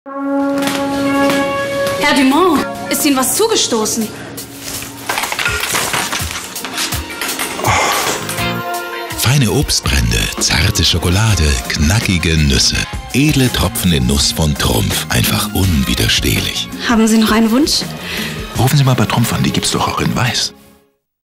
Trumpf - Edle Tropfen (Sky Du Mont) | Werbespot, April 2012
Die detaillierte Beschreibung dieses Werbespots sowie weitere Anmerkungen dazu folgen in Kürze.